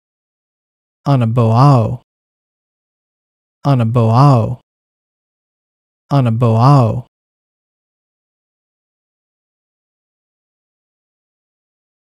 How to pronounce Anaboaō in Biblical Greek - (ἀναβοάω / cry out)
ἀναβοάω (anaboaō) means cry out in Biblical Greek.

This video shows how to pronounce ἀναβοάω in the Erasmian pronunciation system.

Example Bible Verses:

Matthew 27:46
And about the ninth hour Jesus cried out with a loud voice, saying, “Eli, Eli, lema sabachthani?” (that is, “My God, my God, why have you forsaken me?”)